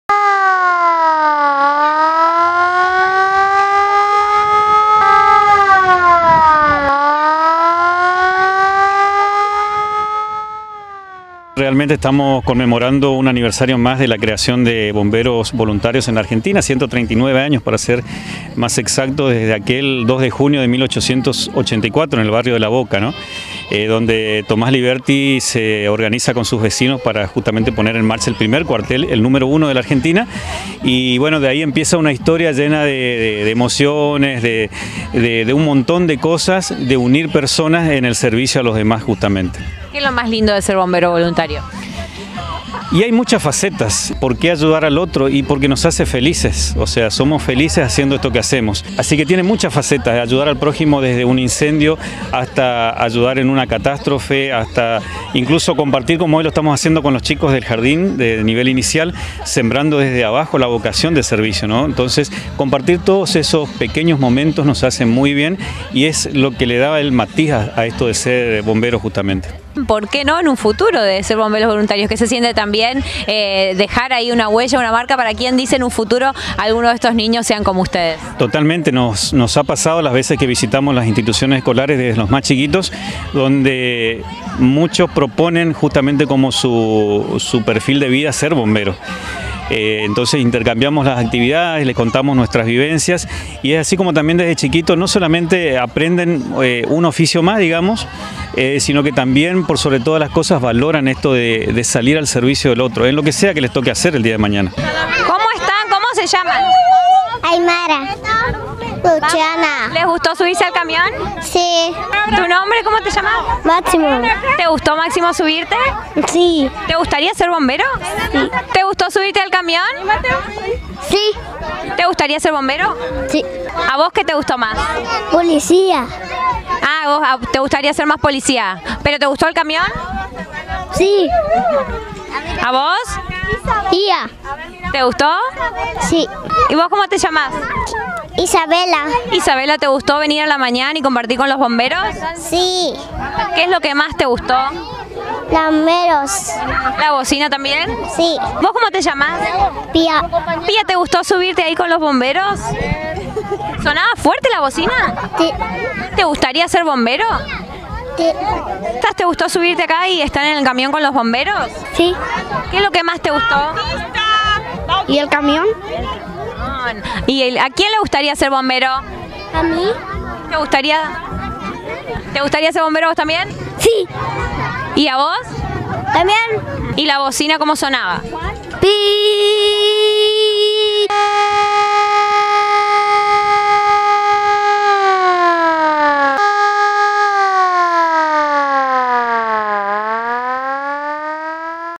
-DIA DEL BOMBERO VOLUNTARIO - NOTICIERO 9
EN CONMEMORACIÓN AL PRIMER CUARTEL CREADO EN LA BOCA
Se conmemora este día en homenaje al primer escuadrón de Bomberos Voluntarios que fue constituido por vecinos autoconvocados del barrio porteño de La Boca. Las cámaras de N9 estuvieron presentes en Puerto Tirol, donde durante todo el día hubo festejos y exposiciones, y nos convertimos en ellos por un dia.
NOTA: NOTA: ROBERTO PEDRAZA - JEFE DEL CUARTEL DE PUERTO TIROL. // ALUMNOS DE SALITAS DE 3,4Y5 /JARDINES DE INFANTES DE TIROL